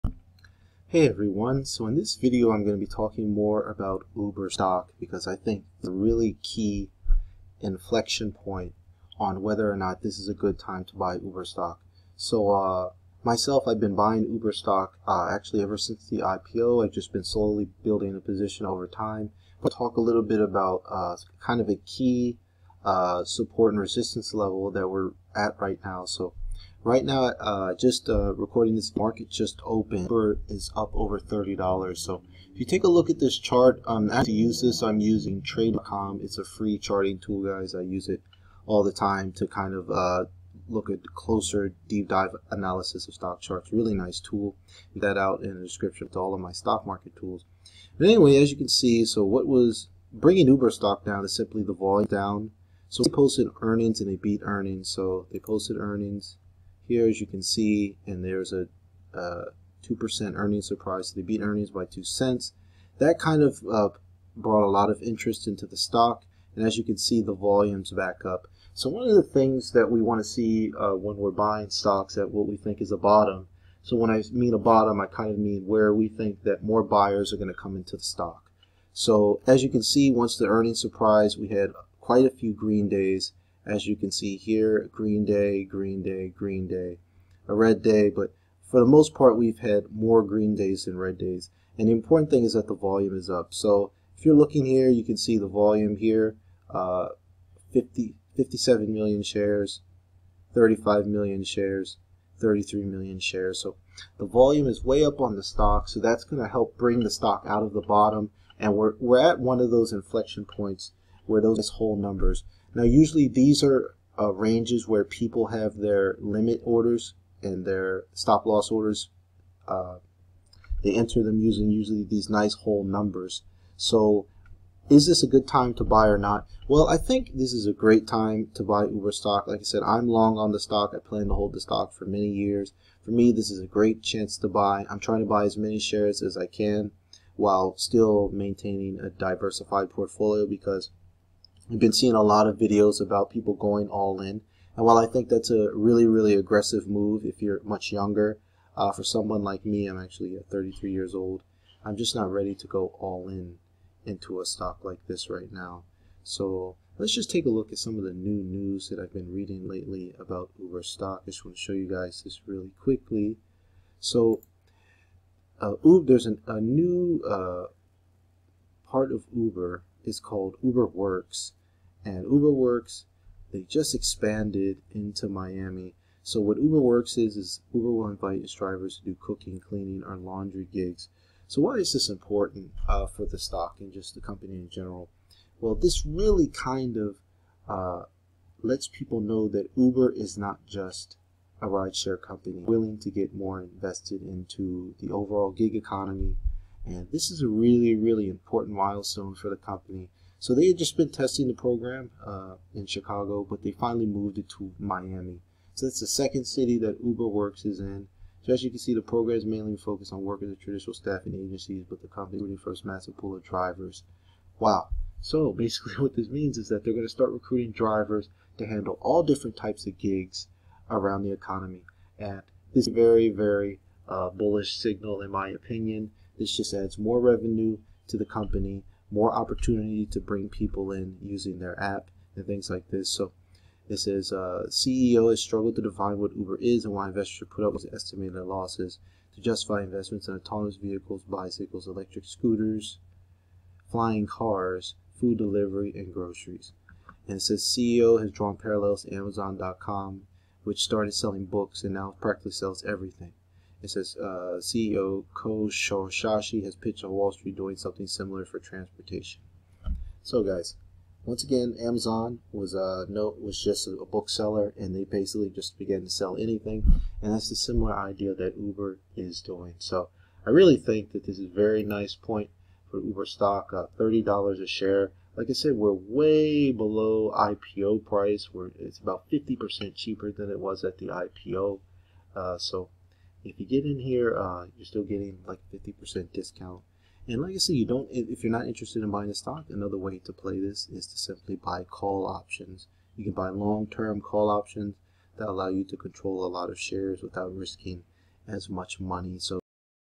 Is it time to buy UBER stock? $30 Now
Is it time to buy Uber stock? Uber just launched their 2nd city for their Uber Works as they attempt to become the dominant player in the gig economy.

The stock just crossed $30, a major inflection point and price level since many traders and investors post limit orders at this range.

Could Uber stock be headed much higher? Only time will tell but this looks like a great opportunity to buy in.